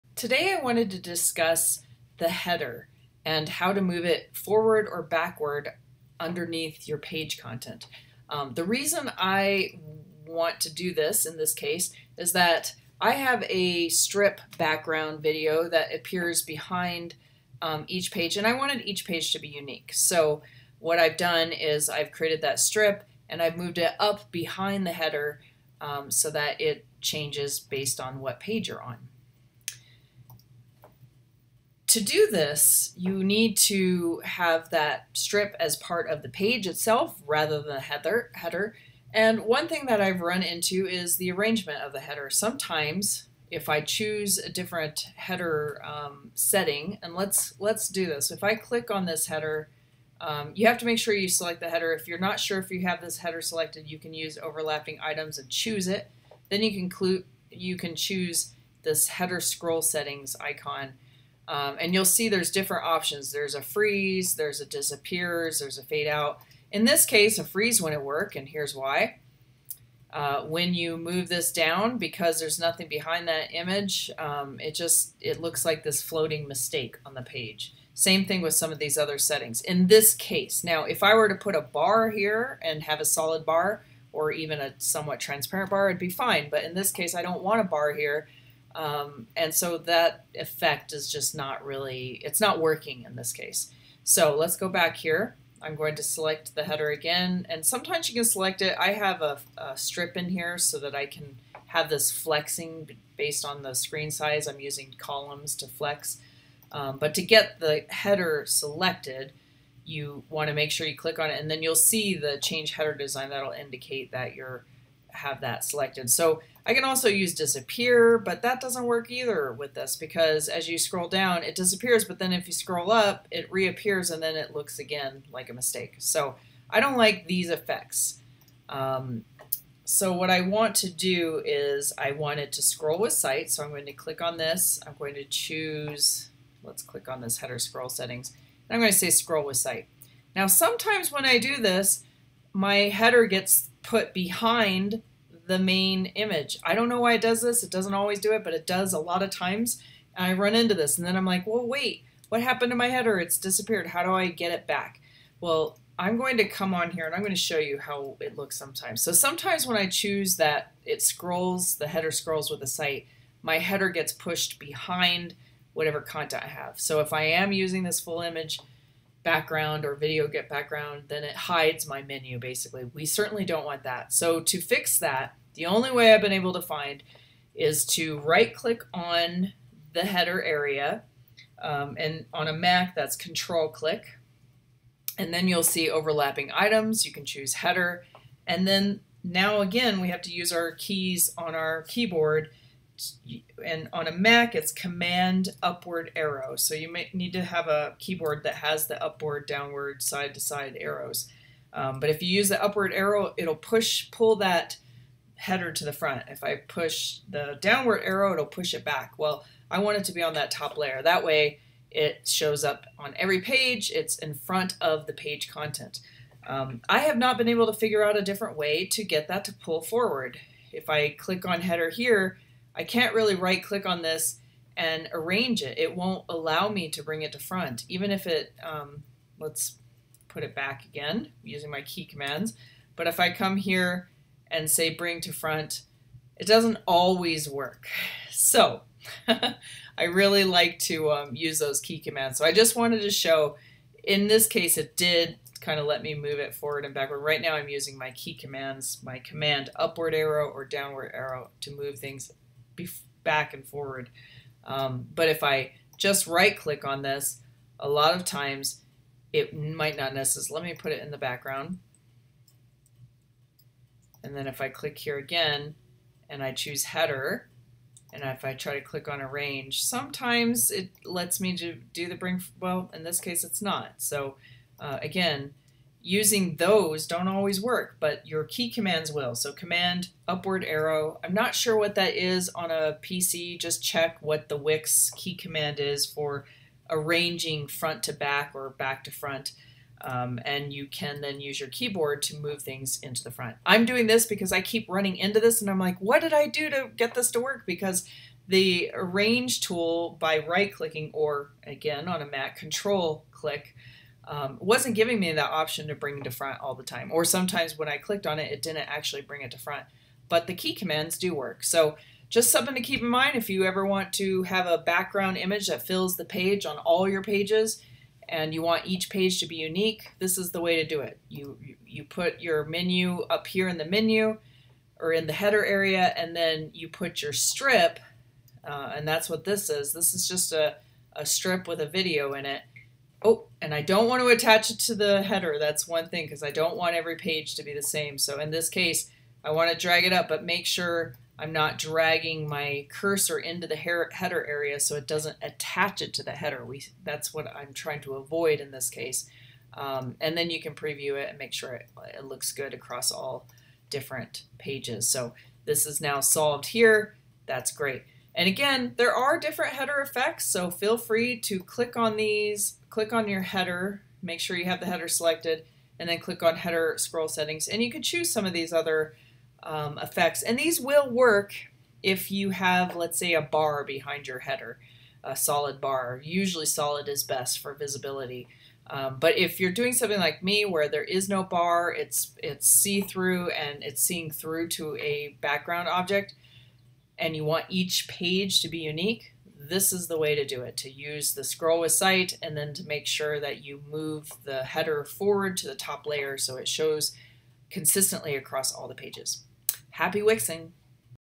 Wix Header Disappeared? Here's how to Bring Forward to overlap Page Content
As a Wix Designer, I keep running into an issue if I try to use a different background video or image on each page – the header gets overlapped by the page content. Here's how to fix this as well as PC key commands